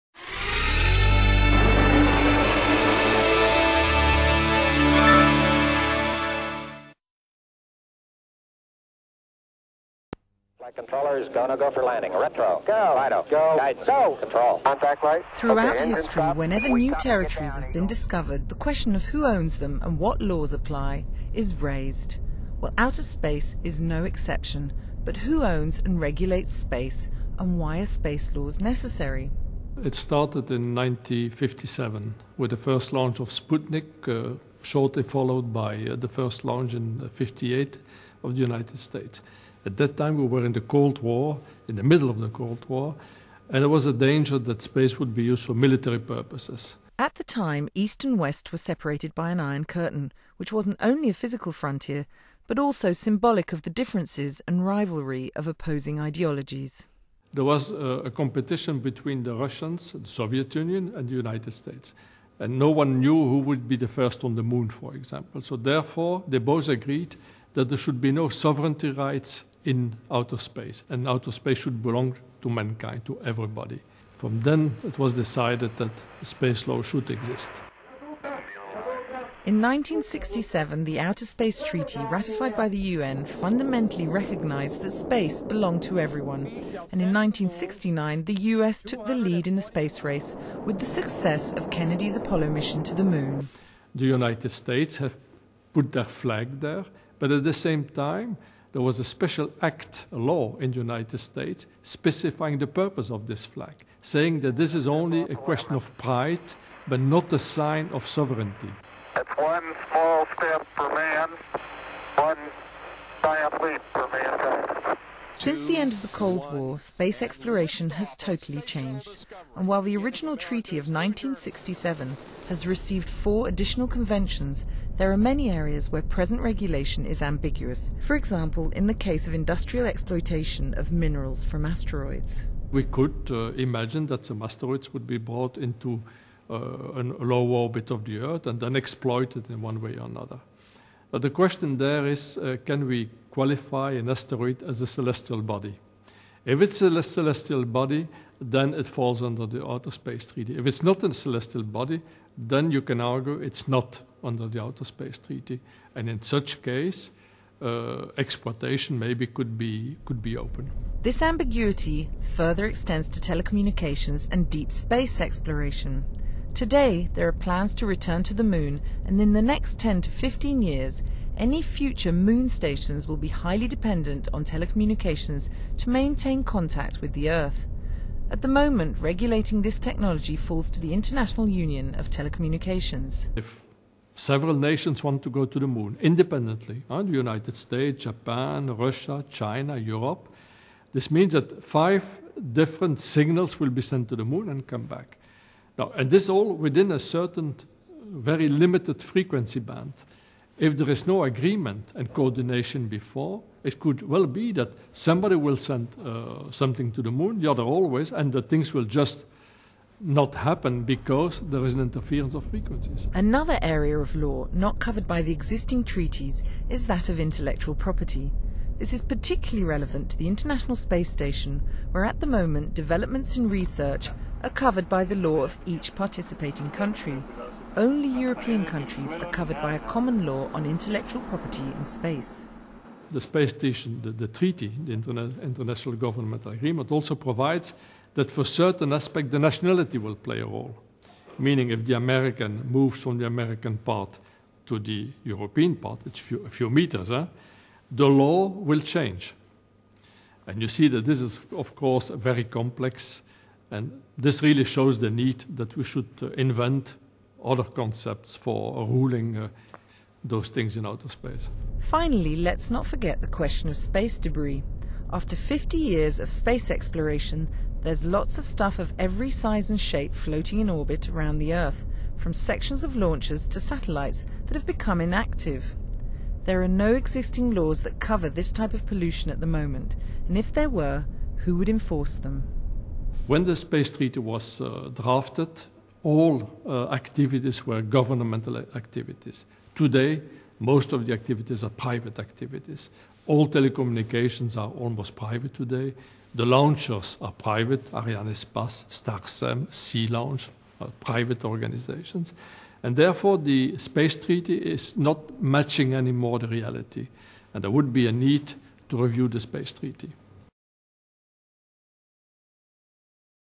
Space Law
Who owns and regulates space, and why are space laws necessary?  via the European Space Agency